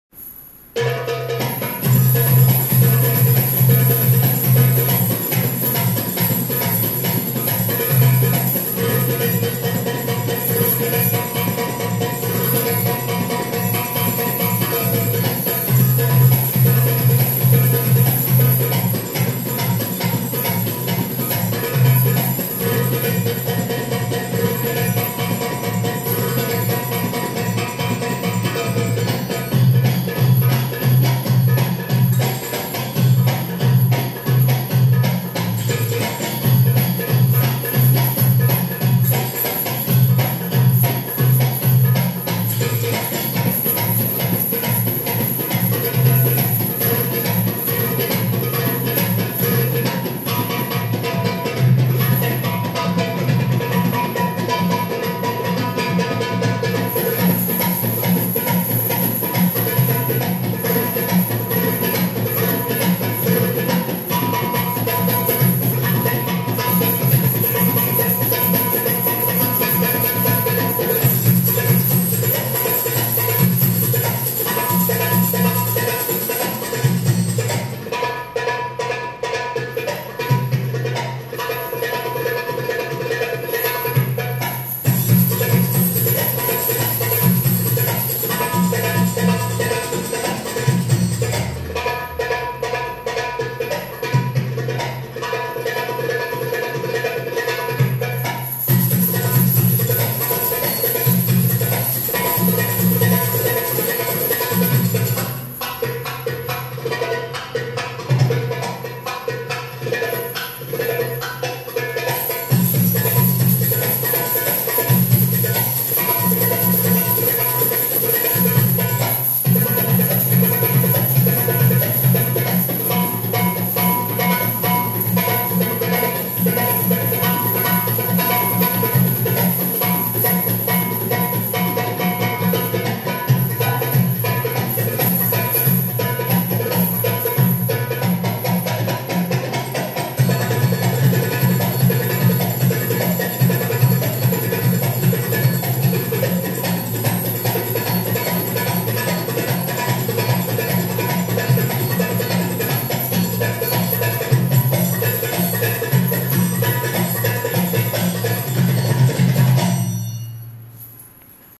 Crystal demonstrating Drum Solo Dolmez in the class 一千零一夜舞坊 Crystal多梅絲鼓舞
Crystal demonstrating Drum Solo Dolmez 一千零一夜舞坊 Crystal多梅絲鼓舞